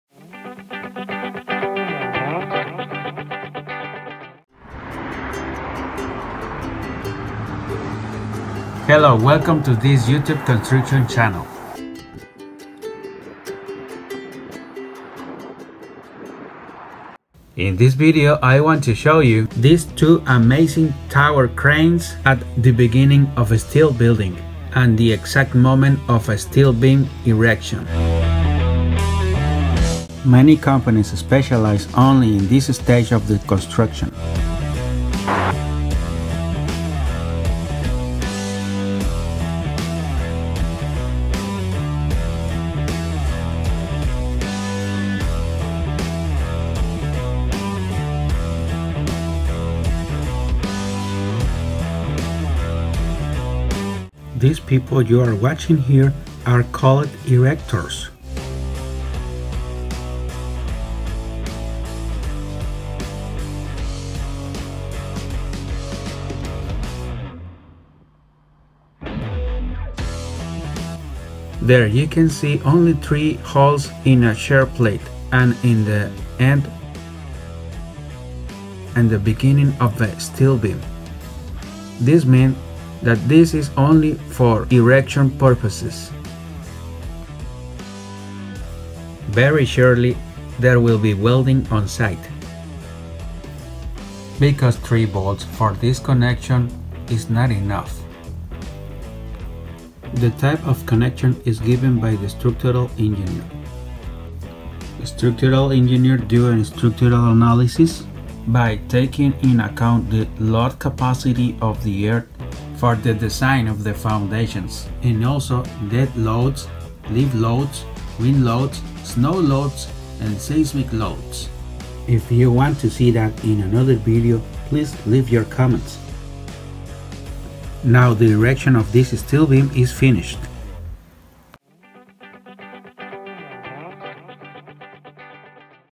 STEEL BUILDING BEAM ERECTION
Hello, how are you friends of this channel? In this video, I bring you the exact time of erection for a heavy steel beam with two four-plate steel columns, it is a building that is beginning and I explain a little about how the structural analysis begins for a steel building. These tower cranes look amazing from the city.